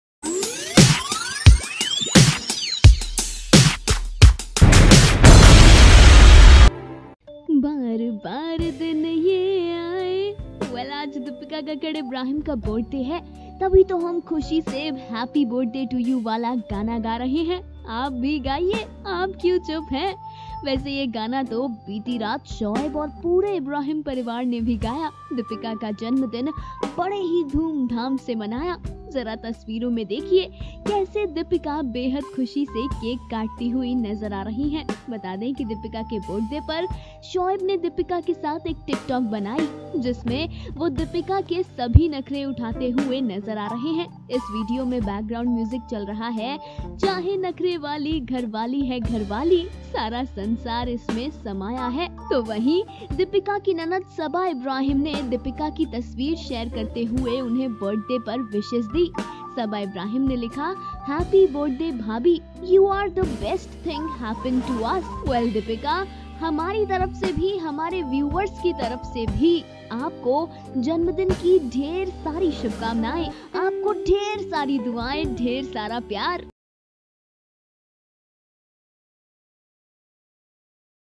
Celebration : Dipika Kakkar Birthday Celebration Inside Video & Pictures | Tik Tok By Shoaika
Dipika Kakar (born 6 August 1986) is an Indian television actress known for playing Simar Bhardwaj in Sasural Simar Ka, winning Bigg Boss 12 and portraying Sonakshi Rastogi in Kahaan Hum Kahaan Tum